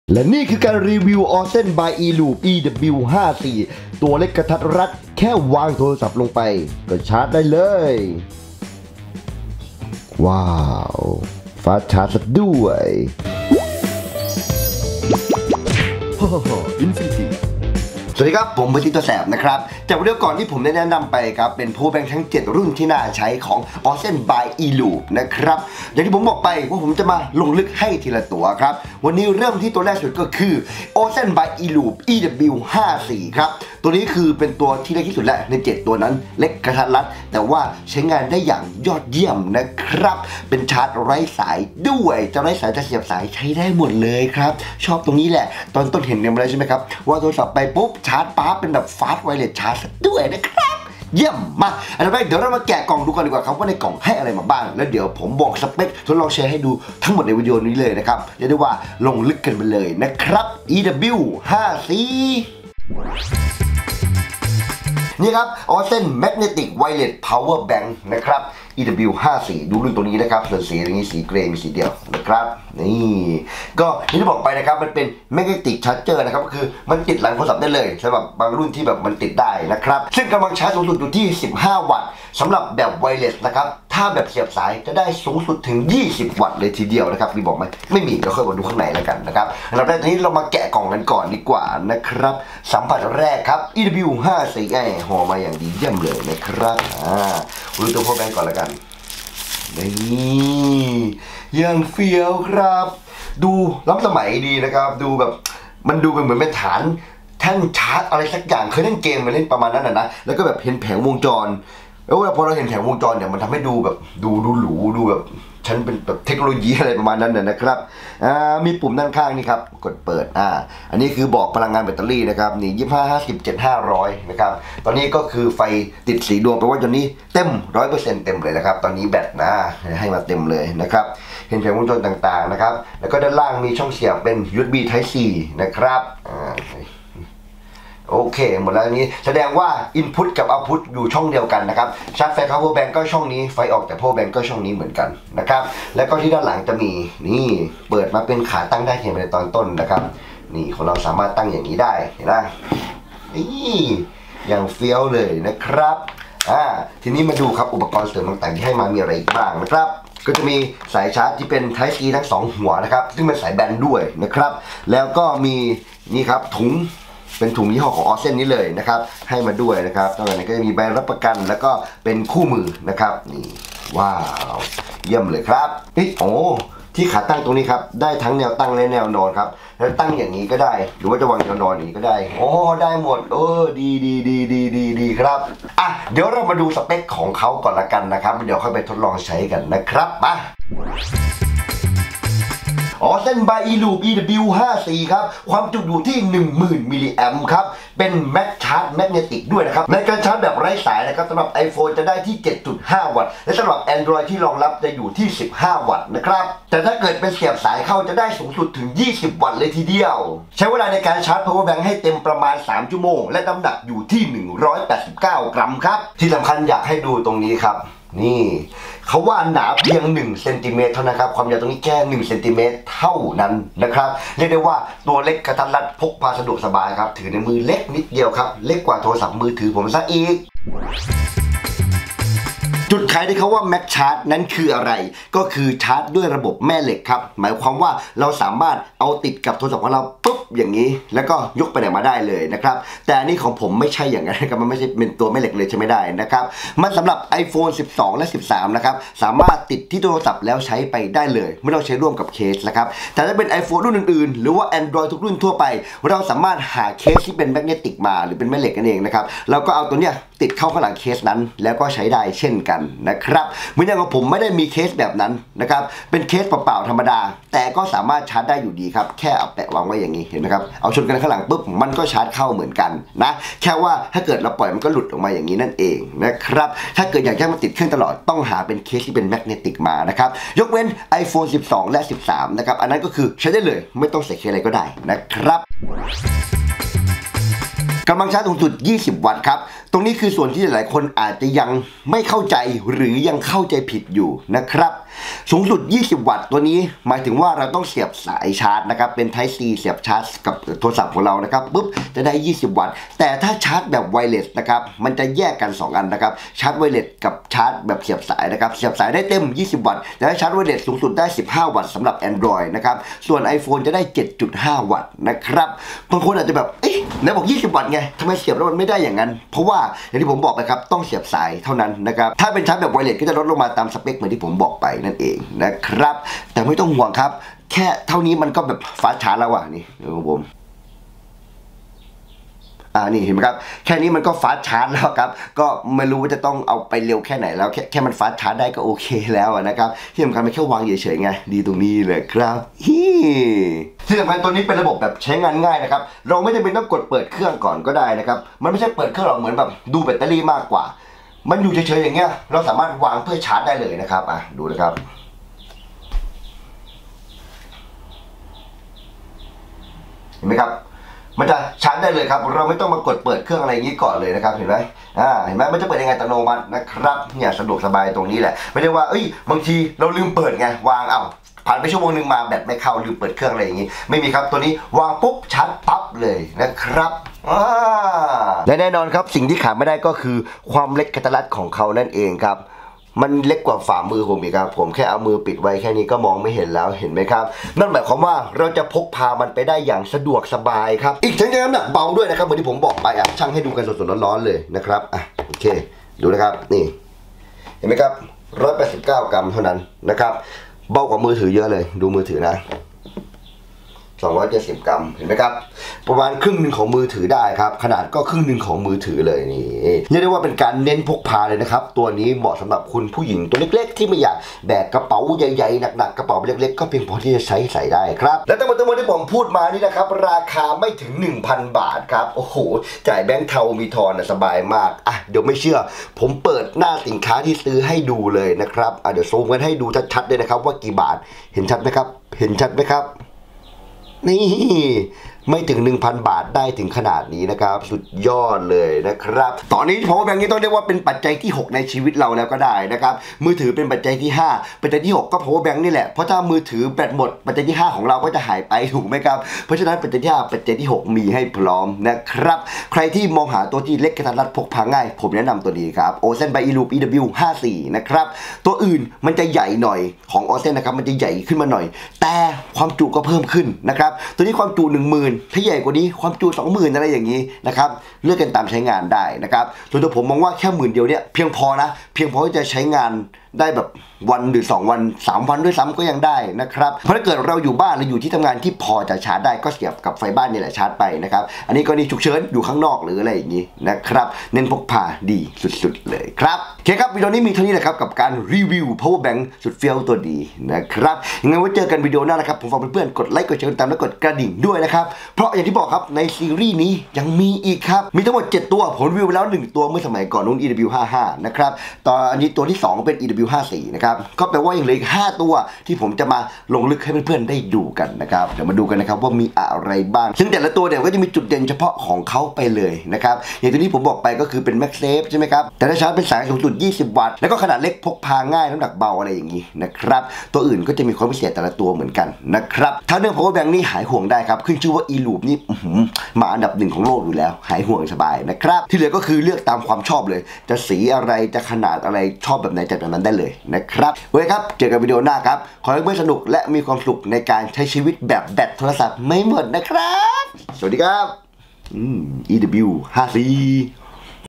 รีวิว ORSEN & Eloop EW54 เล็กกระทัดรัด จุโหดๆ | รีวิวของเจ๋งๆ | เพื่อนซี้ ตัวแสบ 👓
ตัวนี้เป็นตัวไซส์เล็ก กระทัดรัด แต่ความจุให้มามากถึง 10,000 mAh แถมเป็น Magcharge ด้วย พกพาสะดวกใส่กระเป๋าสบาย ใช้ได้ทุกที่ทุกเวลา